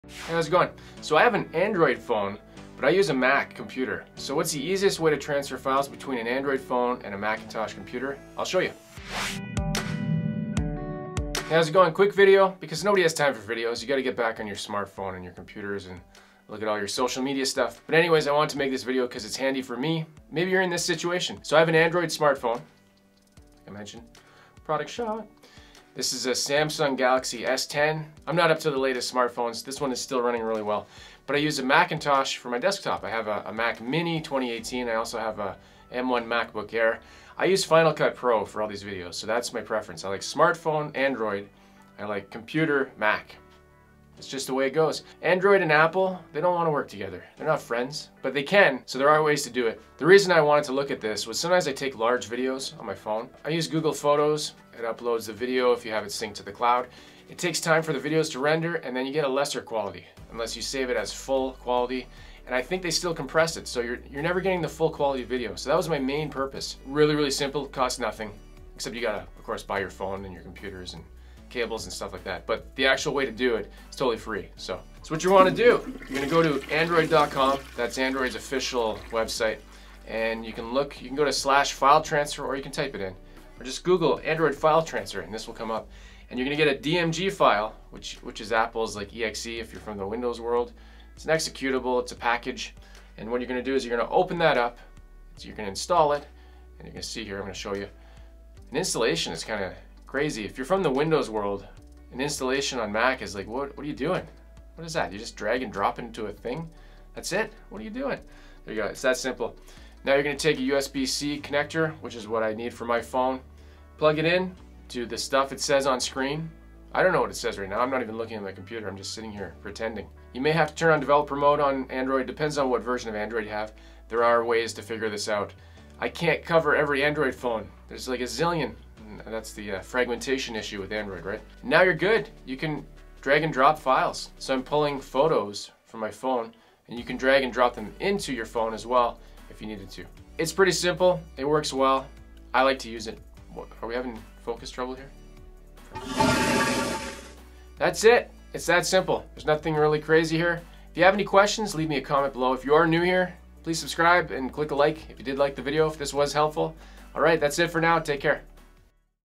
How To Transfer YOUR Android Smartphone Files to a Mac Computer
I have an Android Smartphone (Samsung Galaxy S10), and I use an Apple Mac computer. What’s the easiest way to transfer files between those two systems? I’ll show you

USB-C Cables 3-Pack I use
Timestamps
0:00 - What I'm doing
0:50 - Why I'm doing?
1:15 - How I'm doing?
2:20 - Please focus

Gear Used in my videos:
📷 Cameras
Sony ZV-E10
Sony AX53
🎤 Microphone
Shure VP83